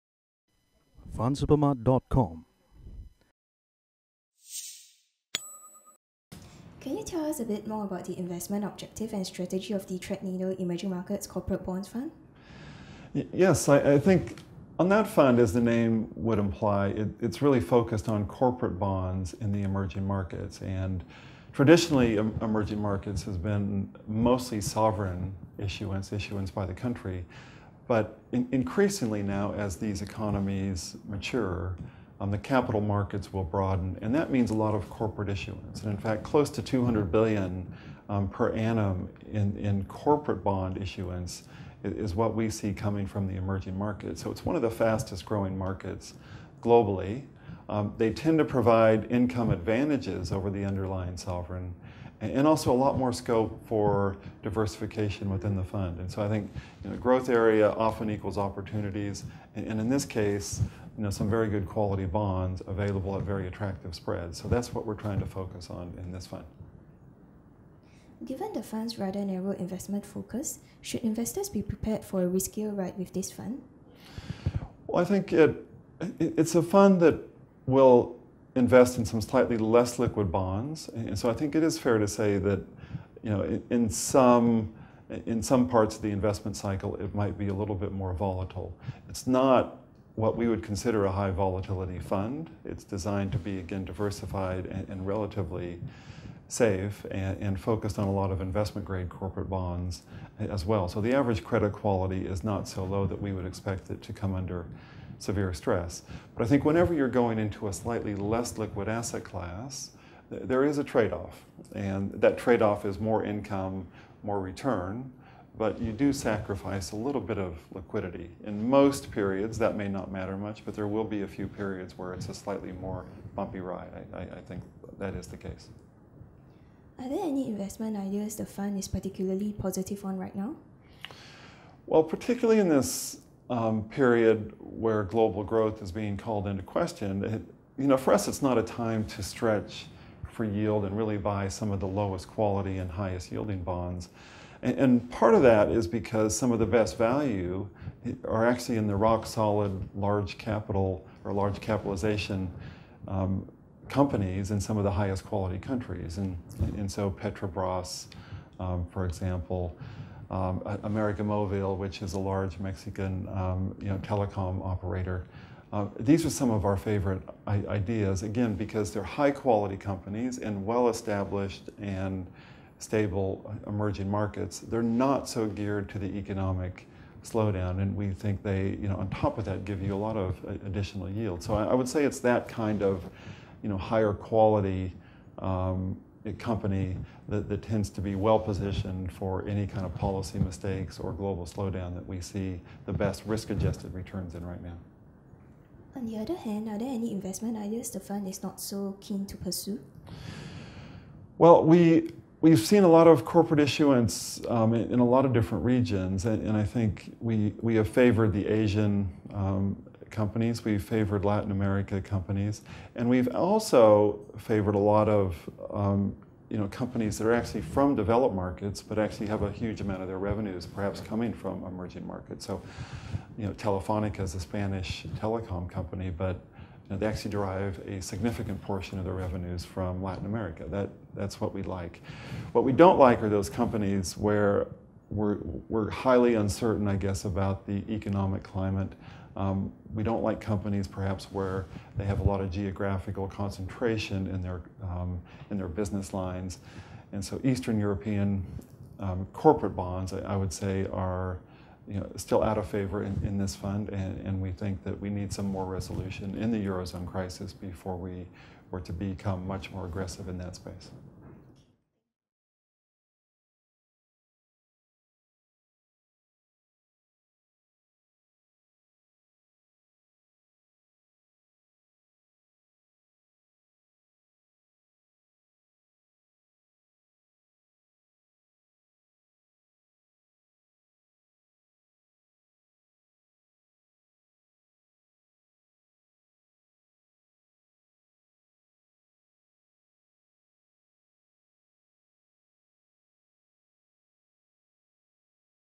Ask The Experts: Greater Income And Diversification Advantages From Emerging Market Corporate Bonds
Jim Cielinski, Head of Fixed Income at Threadneedle Investments shares his outlook for emerging market corporate debt as well as the investment decisions undertaken by the Threadneedle Emerging Market Corporate Bonds Fund.